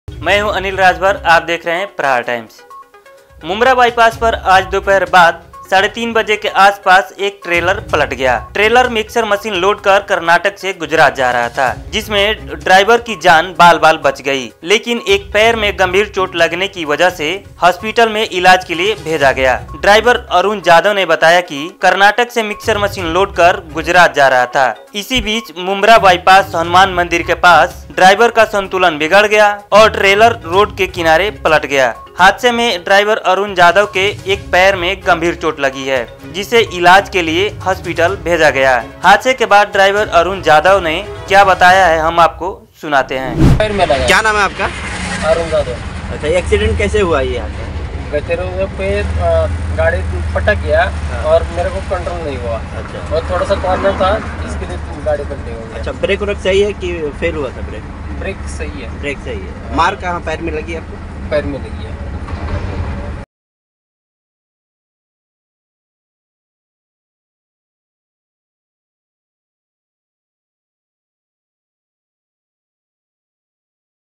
मुम्ब्रा बाईपास पर हादसा,ड्राइवर हुआ घायल || Islamuddin Khan
मुम्ब्रा बाईपास पर आज दोपहर बाद साढ़े 3 बजे के आसपास में एक ट्रेलर पलट गया जिसमे ड्राइवर की जान बाल बाल  बच गई मगर एक पैर में गभीर चोट लगी है जिसे हॉस्पिटल इलाज के लिए भेजा गया है, ड्राइवर अरुण जाधव  ने बताया कि  कर्नाटक से मिक्सर मशीन लोड कर के गुजरात  जा रहा था और मुम्ब्रा बाईपास हनुमान मंदिर के पास ड्राइवर का संतुलन बिगड़ गया और ट्रैलर रोड के किनारे पलट गया हादसे में ड्राइवर अरुण जाधव के एक पैर में चोट लगी हैं इलाज के लिए हॉस्पिटल भेज गयाहै